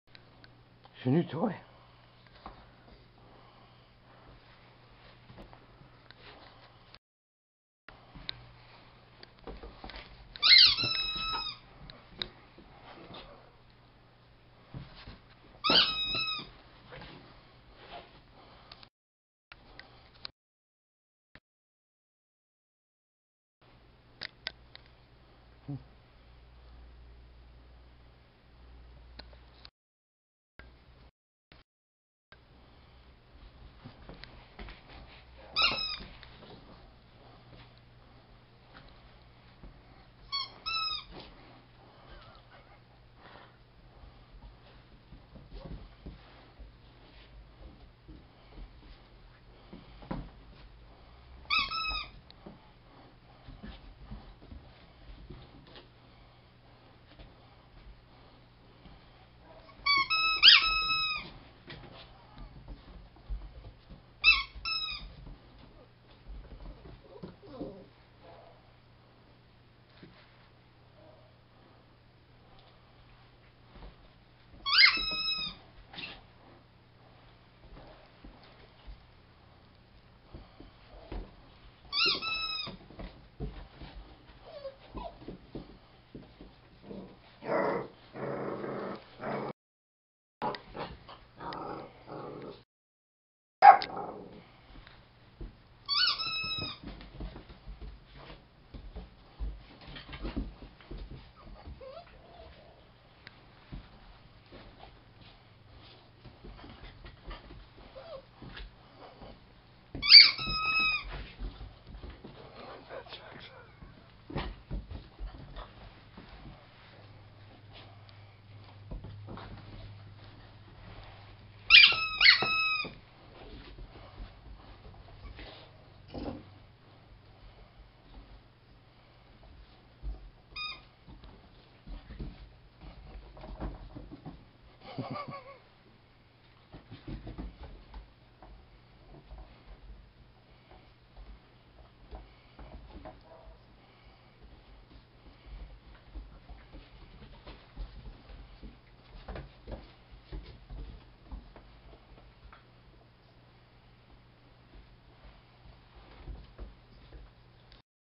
Lucy's New Toy
Lucy got a new squeak toy.  She didn't quite know what to make of it.